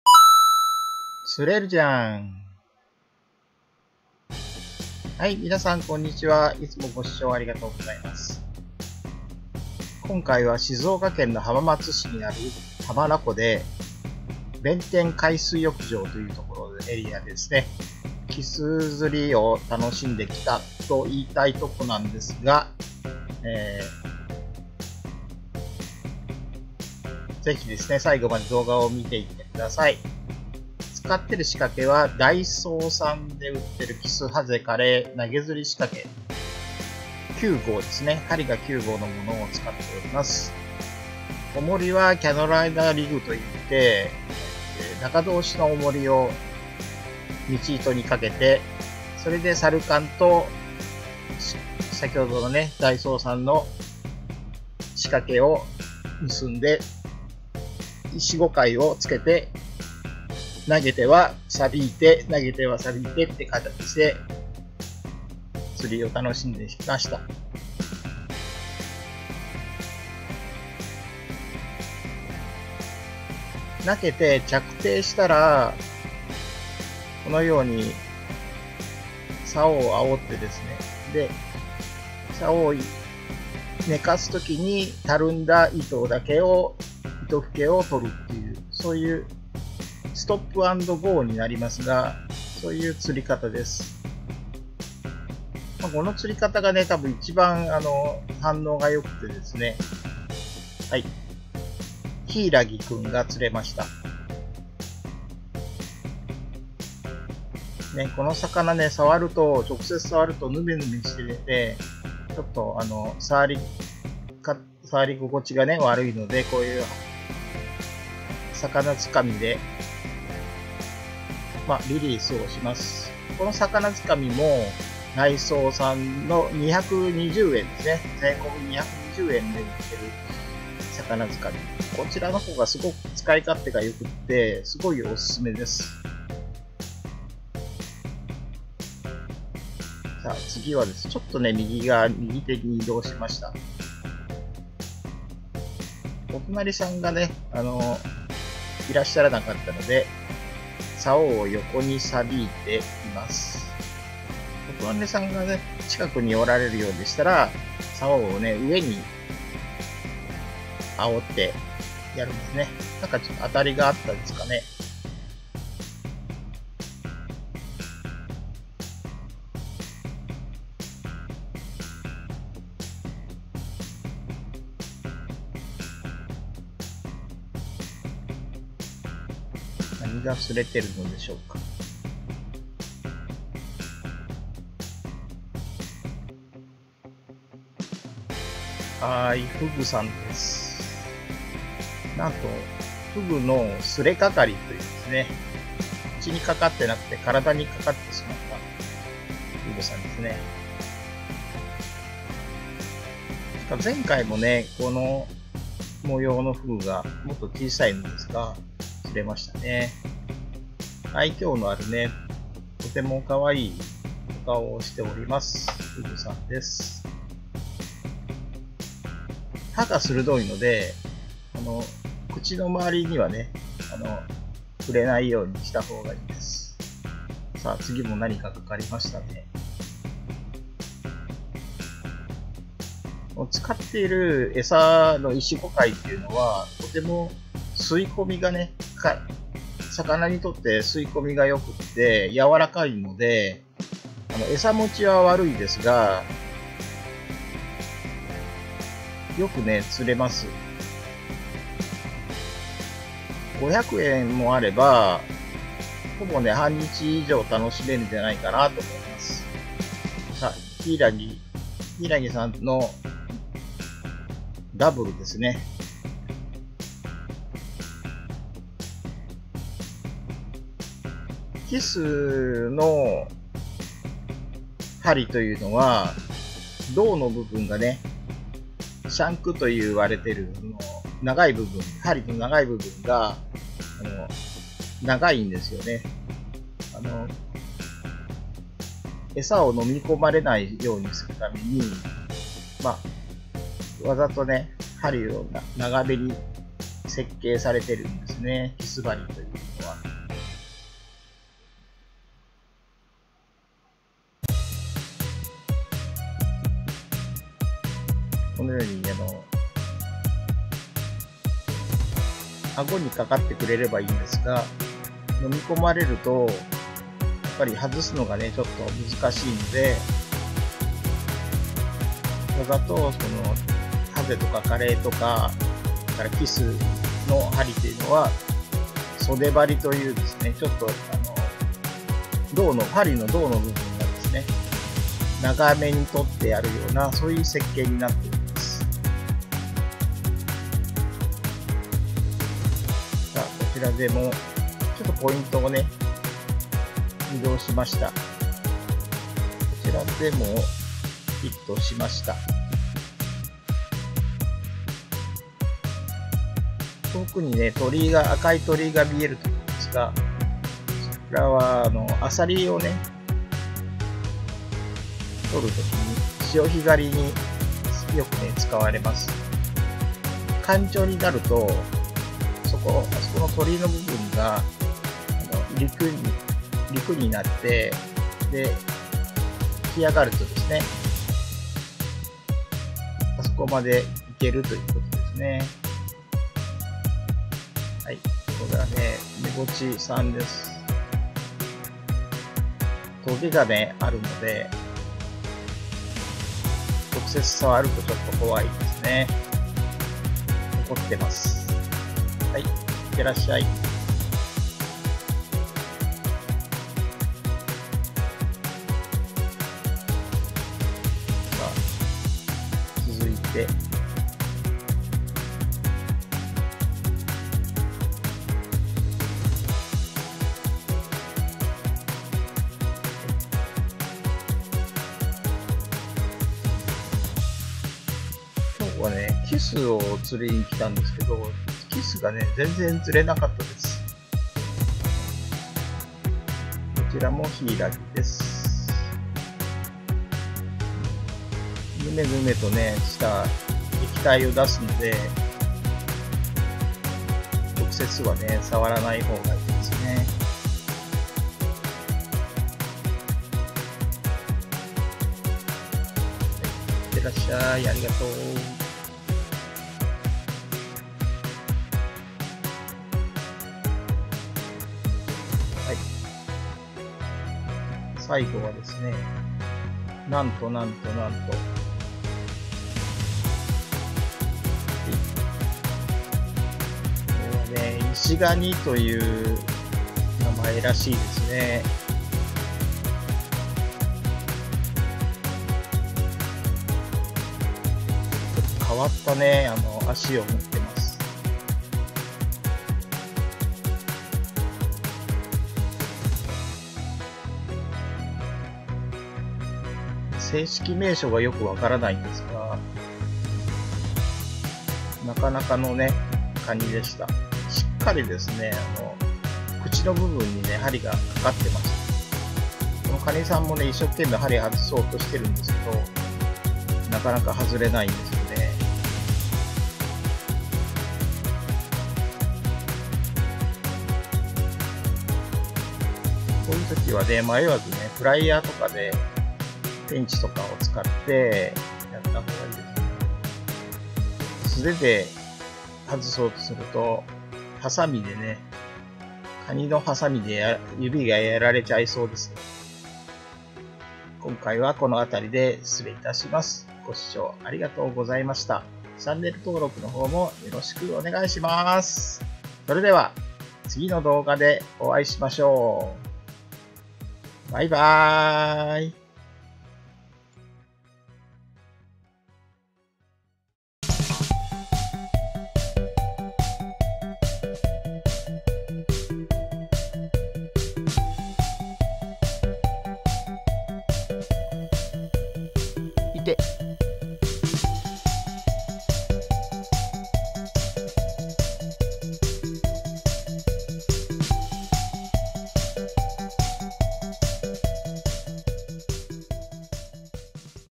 お手軽だけど奥が深いチョイ投げ釣り♬ 浜名湖弁天海水浴場
チョイ投げ釣り♬　浜名湖弁天海水浴場

今回は静岡県の浜名湖の弁天海水浴場でチョイ投げ釣りを楽しんできました。
宜しければ最後までご覧くださいませ。